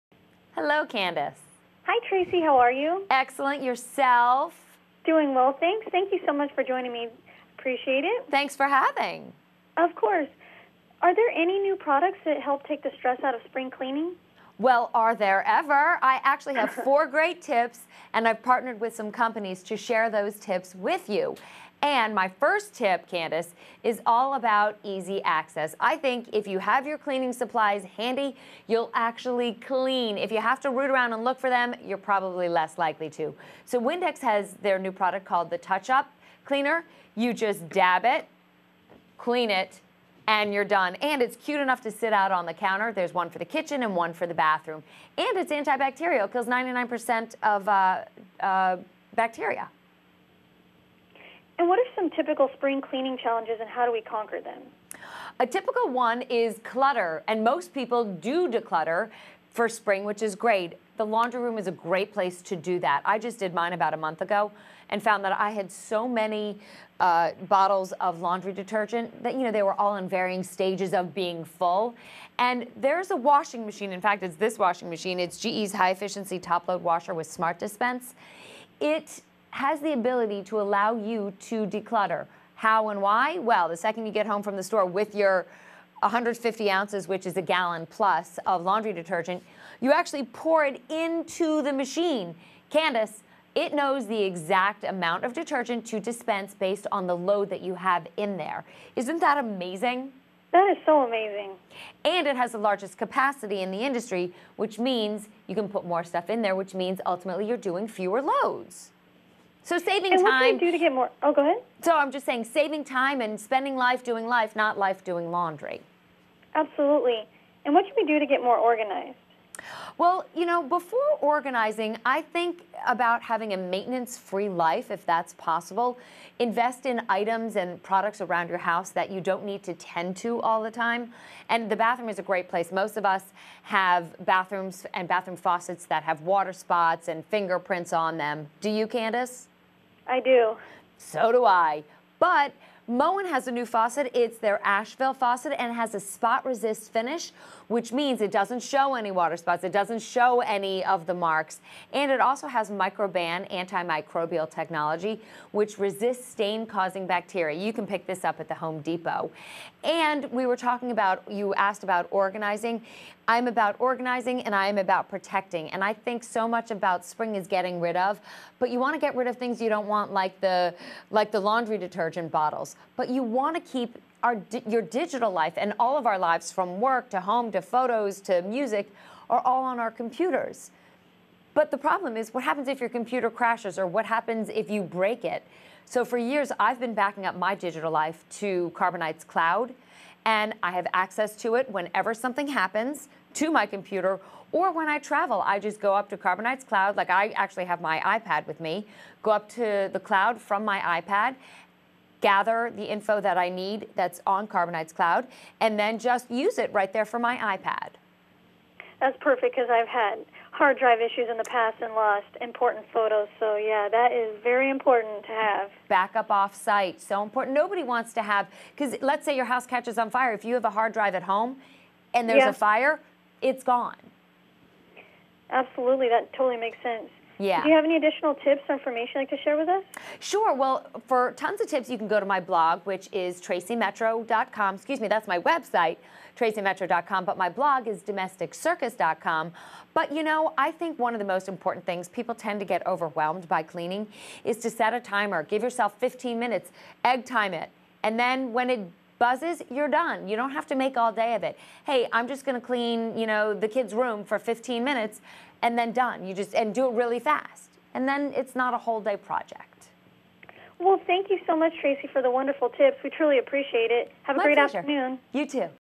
Lifestyle expert Tracy Metro shares spring cleaning and decluttering tips!
Lifestyle expert Tracy Metro joined Candace Rose of CandieAnderson.com to share tips on how to spring clean, declutter and organize with ease! Link to post